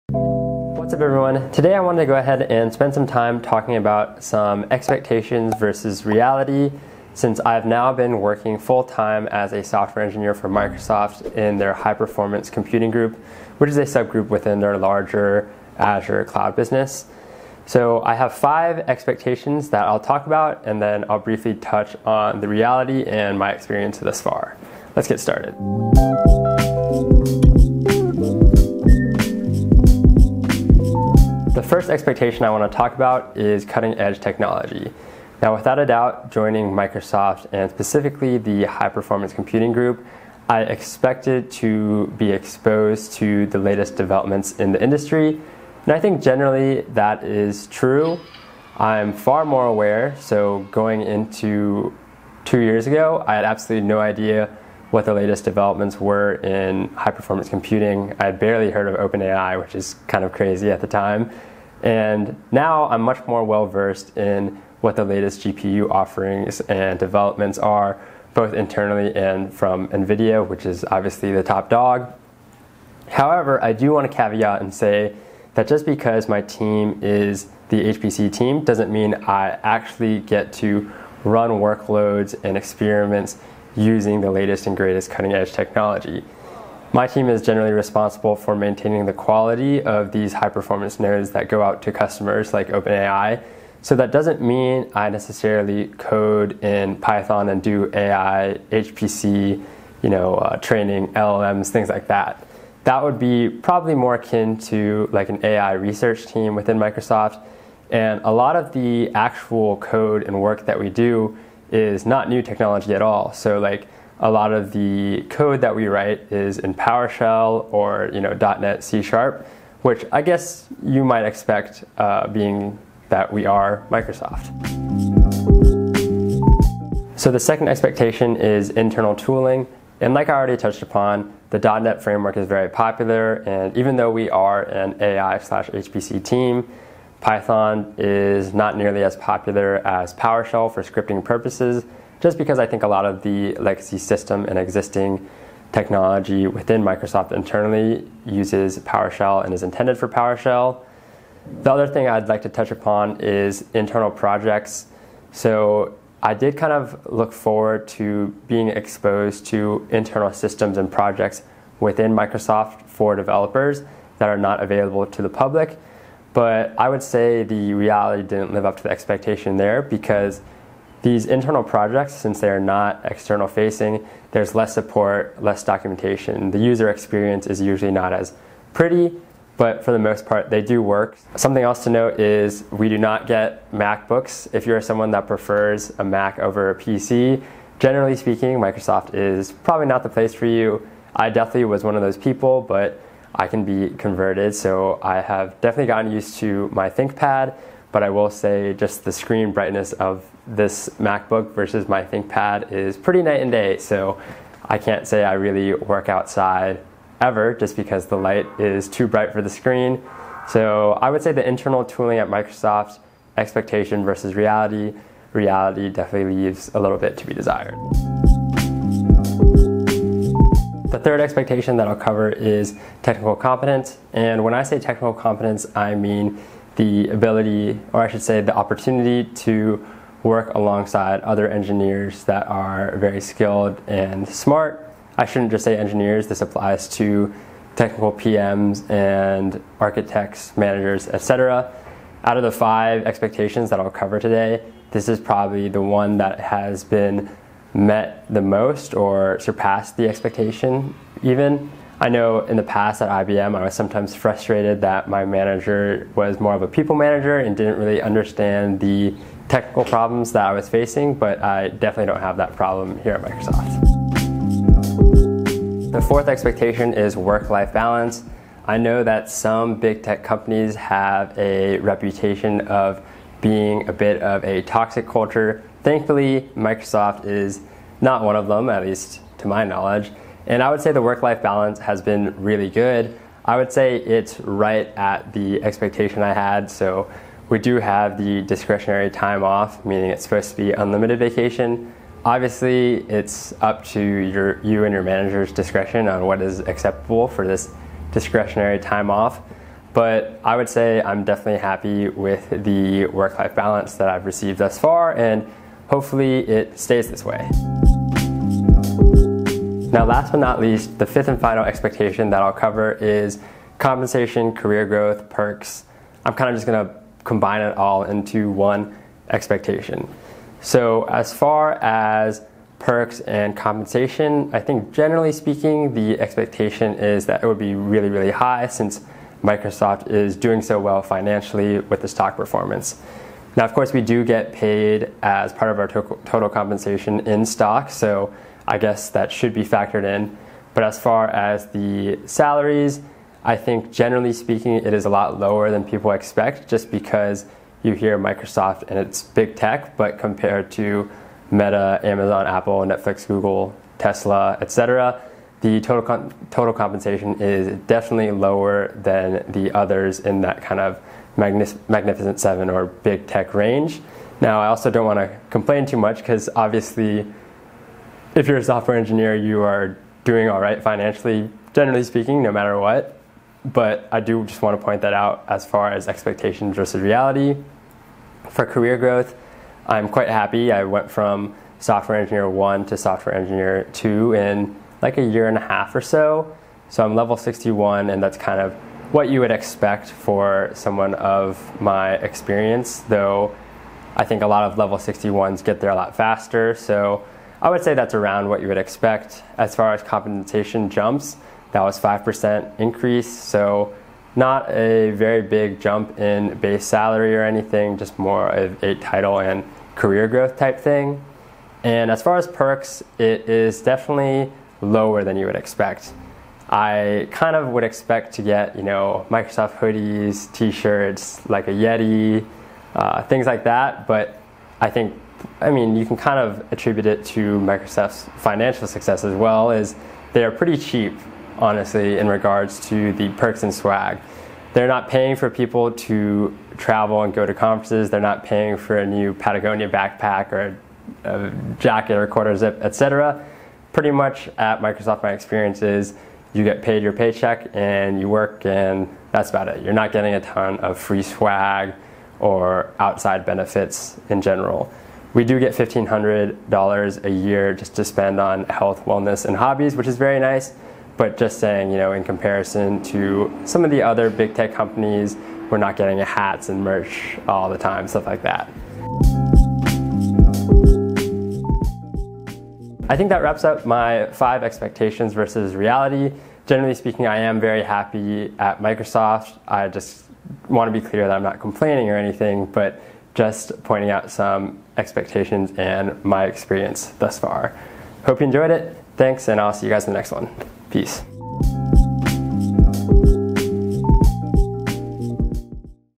Realities of Software Engineering at Microsoft (Azure/HPC)
I wanted to make a quick video comparing my expectations when joining Microsoft- specifically Azure's high performance computing (AI/HPC) group. Hope you enjoyed! Thanks.

CHAPTERS
00:00 Intro
00:32 Technology
02:05 Internal Tooling
03:39 Technical Competence
04:25 Work-Life Balance
05:10 Compensation, Perks, Career Growth
07:53 Wrap-Up